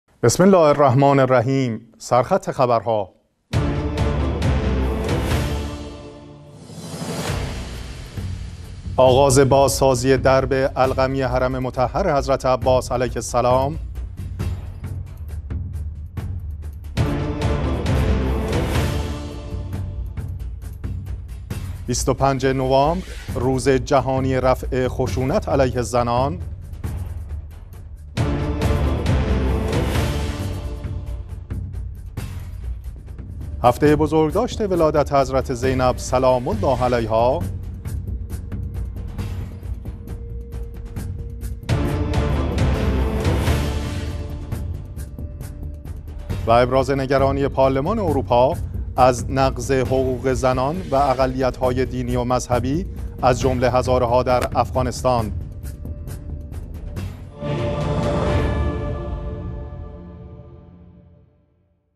سرخط خبرها: شنبه 5 آذر ماه 1401
۱- آغاز بازسازی درب علقمی حرم مطهر حضرت عباس علیه السلام
۲- گزارش ویژه بمناسبت روزجهانی رفع خشونت علیه زنان
۳-  هفته بزرگداشت ولادت حضرت زینب سلام الله علیها
۴- ابراز نگرانی پارلمان اروپا از نقض حقوق زنان و اقلیت‌های دینی و مذهبی از جمله هزاره ها در افغانستان
۵- ادامه اجرای پروژه "تخریب مساجد اسلامی هند"
۶- تشرف رهبران مسیحی سوریه و لبنان به حرم حضرت علی علیه السلام
7- به یاد فتوای تاریخی میرزای شیرازی